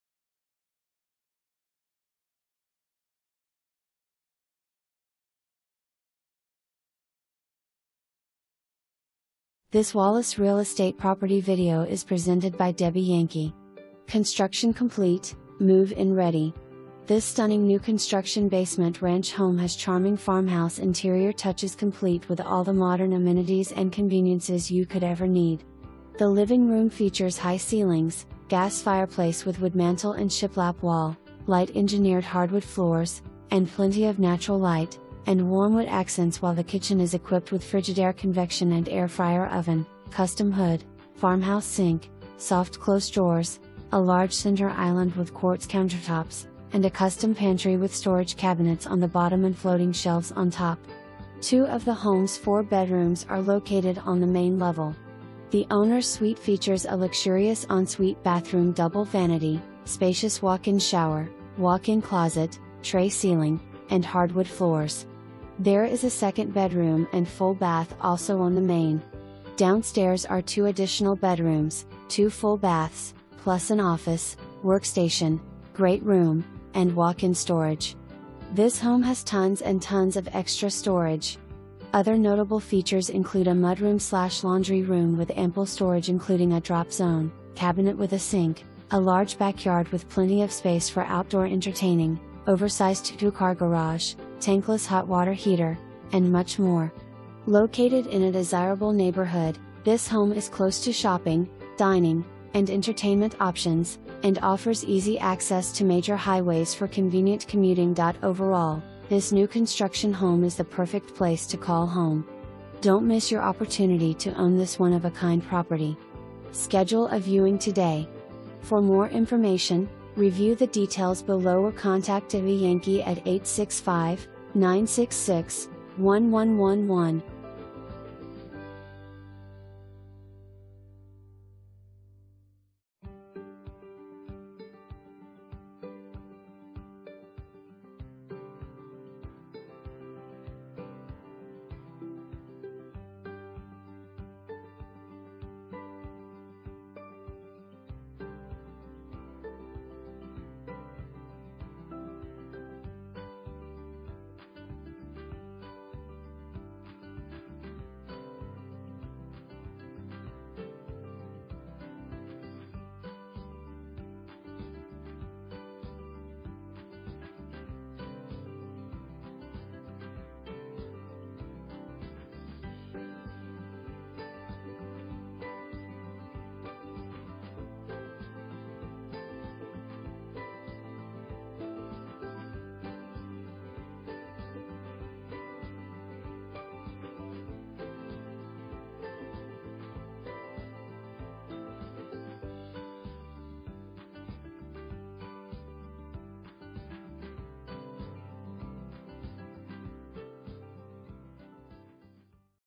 Wallace Real Estate - 415 Alpine Drive
Construction Complete - Move-in Ready!!! This stunning new construction basement ranch home has charming farmhouse interior touches complete with all the modern amenities and conveniences you could ever need. The living room features high ceilings, gas fireplace with wood mantle and shiplap wall, light engineered hardwood floors, and plenty of natural light, and warm wood accents while the kitchen is equipped with Frigidaire convection and air fryer oven, custom hood, farmhouse sink, soft-close drawers, a large center island with quartz countertops, and a custom pantry with storage cabinets on the bottom and floating shelves on top. Two of the home's 4 bedrooms are located on the main level. The owner's suite features a luxurious en-suite bathroom double vanity, spacious walk-in shower, walk-in closet, trey ceiling, and hardwood floors. There is a second bedroom and full bath also on the main. Downstairs are two additional bedrooms, two full baths, plus an office, workstation, great room, and walk-in storage. This home has tons and tons of extra storage. Other notable features include a mudroom/laundry room with ample storage including a drop-zone, cabinet with a sink, a large backyard with plenty of space for outdoor entertaining, oversized two-car garage, tankless hot water heater, and much more. Located in a desirable neighborhood, this home is close to shopping, dining, and entertainment options, and offers easy access to major highways for convenient commuting.Overall, this new construction home is the perfect place to call home. Don't miss your opportunity to own this one-of-a-kind property! Schedule a viewing today!

4 full bath

Debbie Yankey
Wallace Real Estate